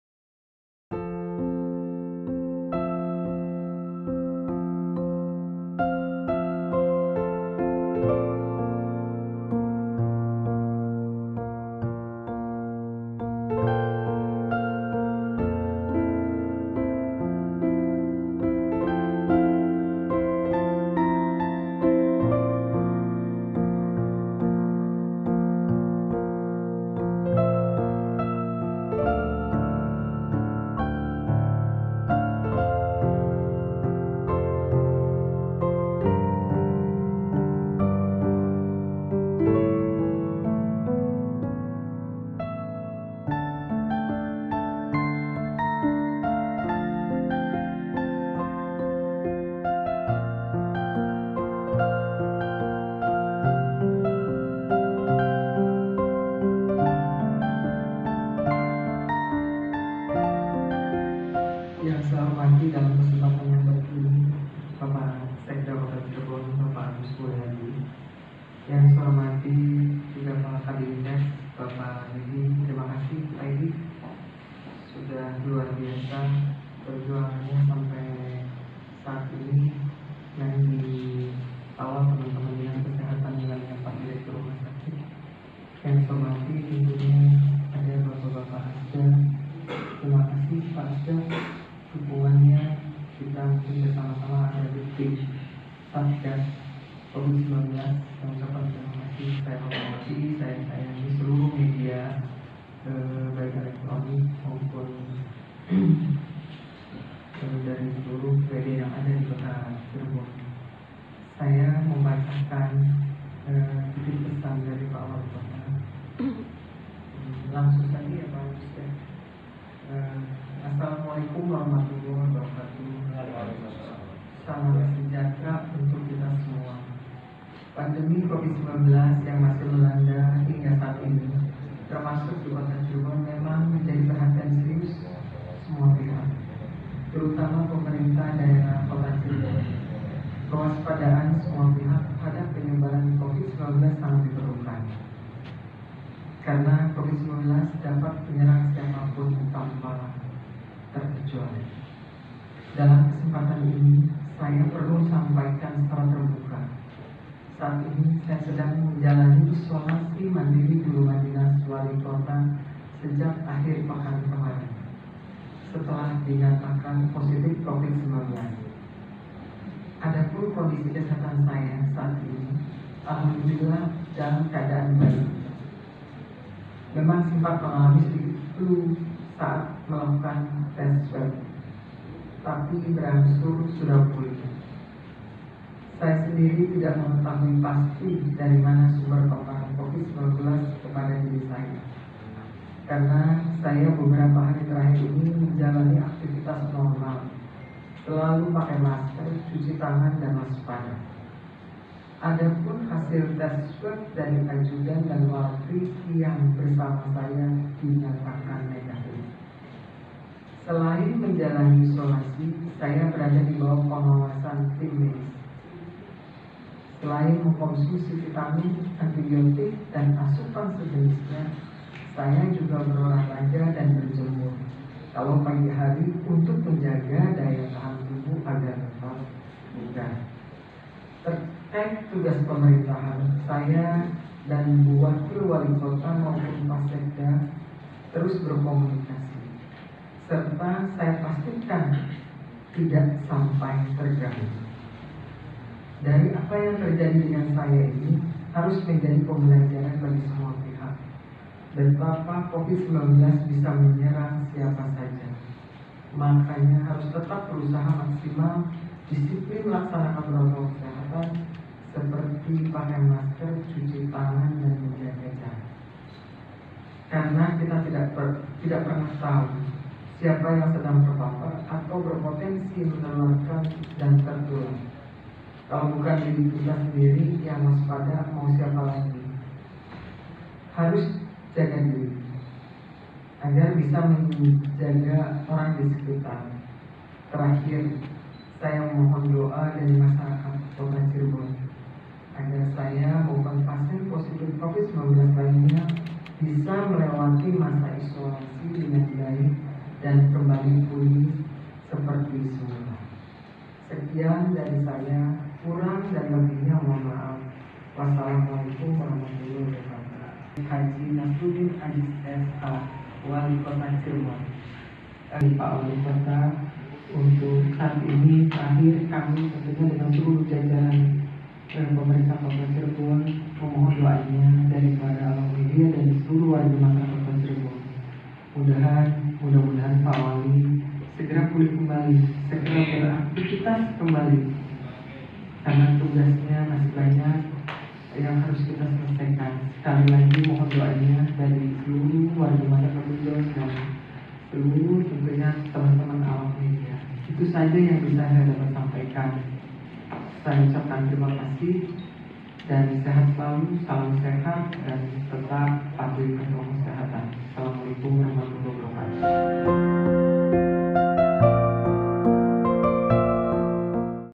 Walikota Cirebon Positif Terinfeksi Covid-19
Walikota Cirebon, Drs H Nashrudin Azis SH terkonfirmasi positif Covid-19, Senin (23/11). Hal tersebut diungkapkan Wakil Walikota Dra Hj Eti Herawati yang membacakan pesan tertulis walikota. Saat ini, walikota sedang menjalani isolasi mandiri dan dalam keadaan baiK..